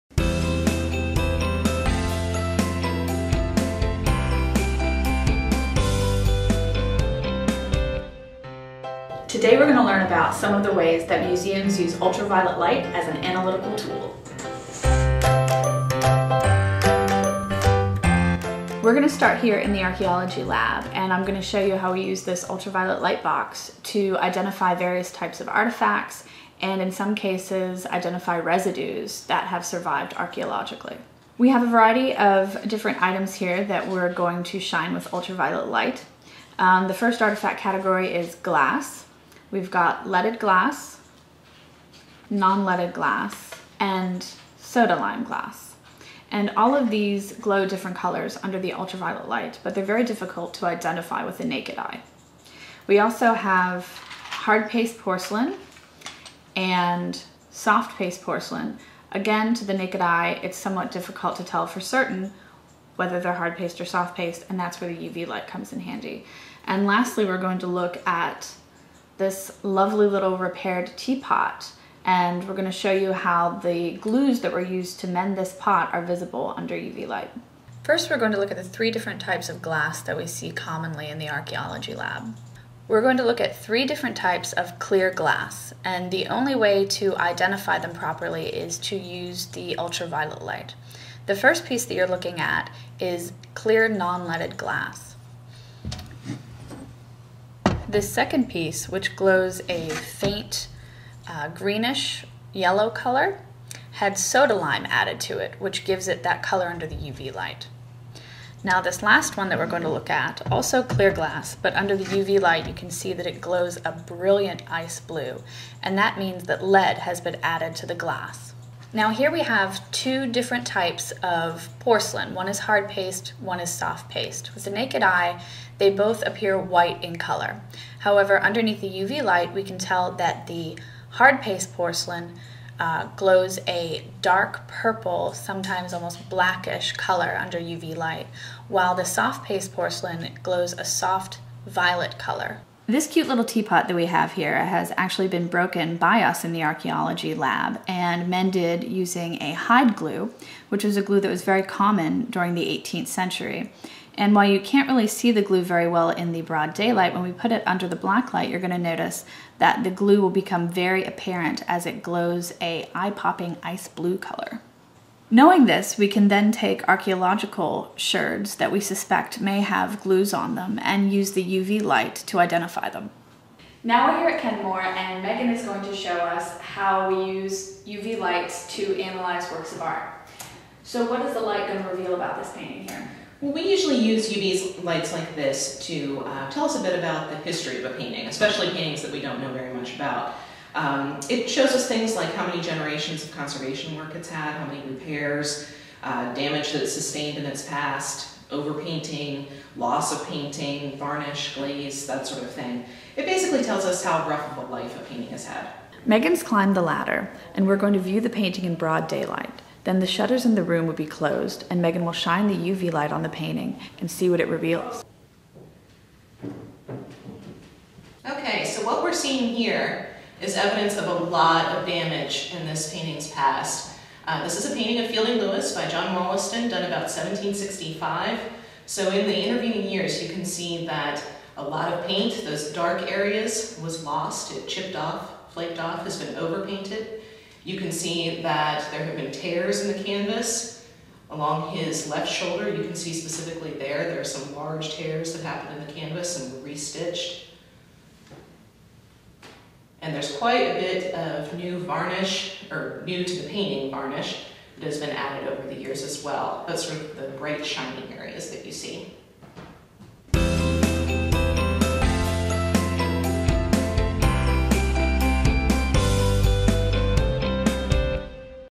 UV Light in Museums
Ultraviolet light is an important and useful tool within the museum world. In this video, we show you how archaeologists and curators use UV light in their work with artifacts and historic objects.